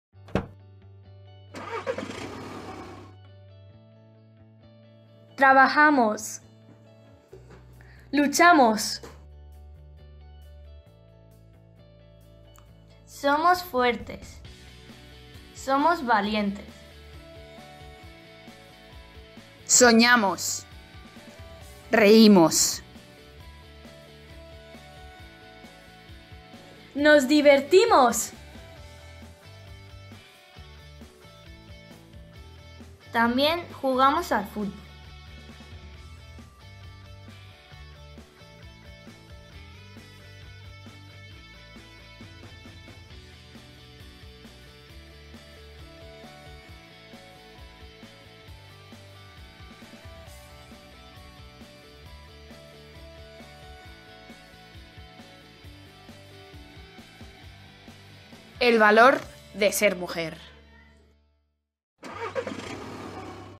El valor de ser mujer
Trabajamos, luchamos, somos fuertes... Las mujeres soñamos con un mundo mejor. ¿Nos ayudas a conseguirlo?

Col·legi Mirasan 1 ESO A
Festival de Clipmetrajes de Manos Unidas